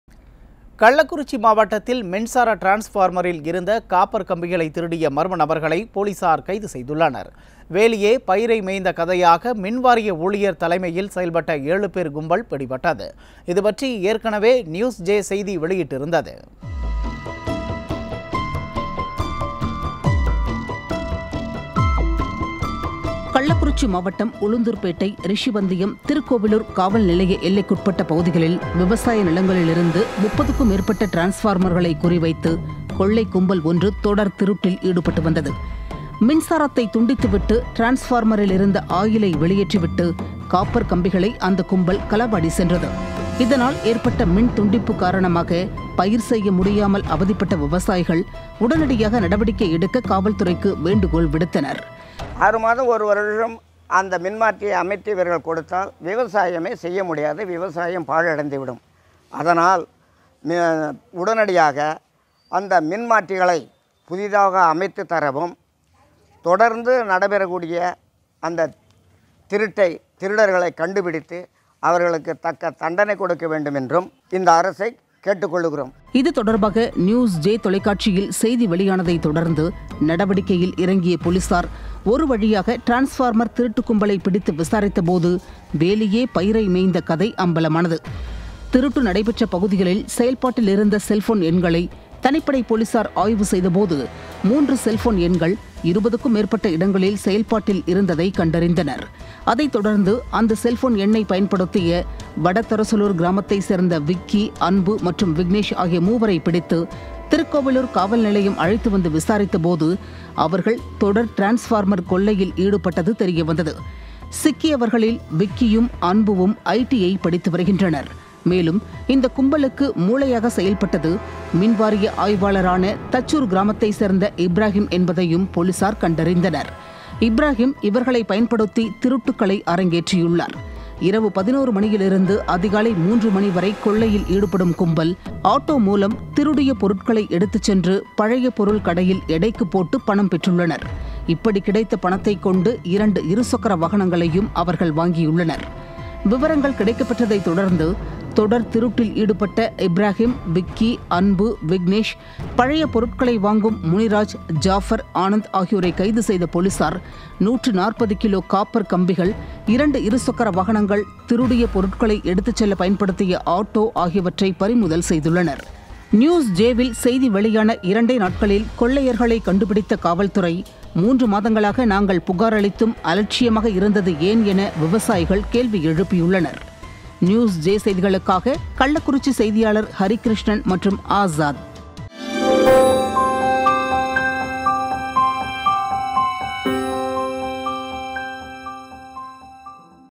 மின்சார டிரான்ஸ்பார்மர் திருட்டு கும்பல் கைதுமின்வாரிய ஆய்வாளர் உள்பட 7 பேரை கைது செய்த போலீசார்
மின்சார டிரான்ஸ்பார்மர் திருட்டு கும்பல் கைதுமின்வாரிய ஆய்வாளர் உள்பட 7 பேரை கைது செய்த போலீசார் | NewsJ